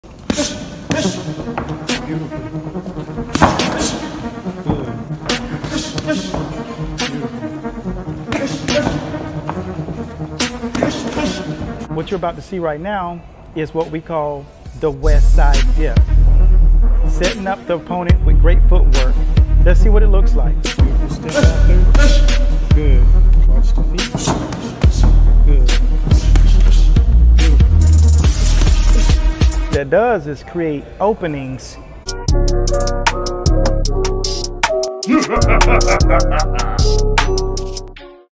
Jersey Joe Walcott | PeekaBoo Boxing
Jersey Joe Walcott was famous for the angles in which he used while standing directly in front of his opposition. This is the west side dip and as my uncle who grew up with this guy says, you can’t hit the man who’s setting traps for you to swing, you’re playing right into his hand. There were a multitude of Learn the peekaboo styles I which were formed.

Learn the variations of the peekaboo in our digital download series simply by clicking